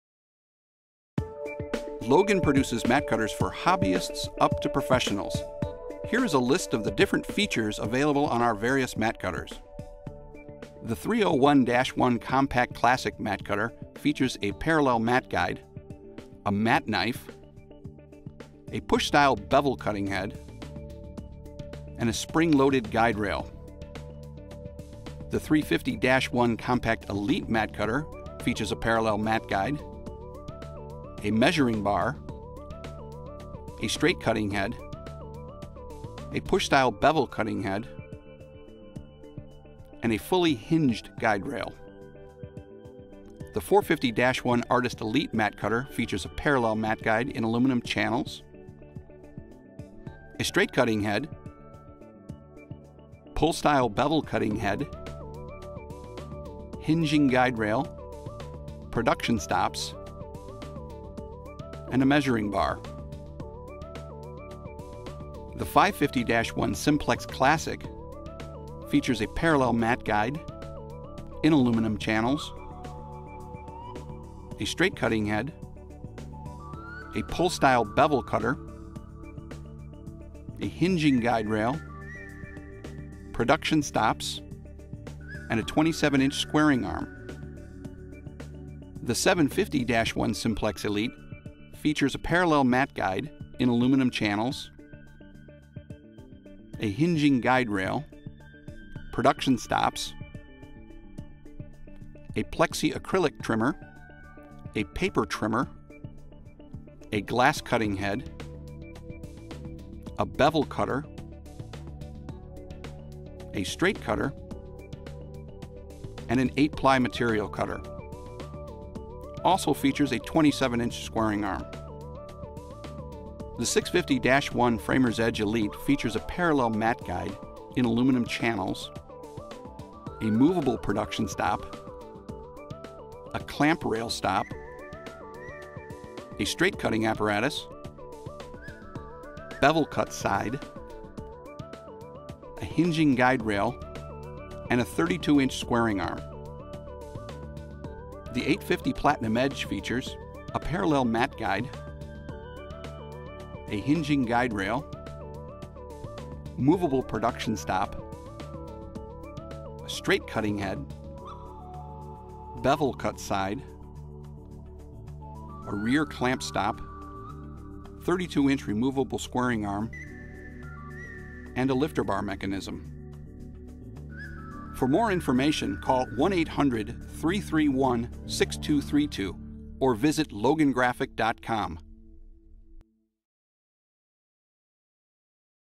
Choosing the Right Logan Mat Cutter
A brief video entitled Choosing the Right Logan Mat Cutter showing the differences between Logan's various board mounted mat cutting systems.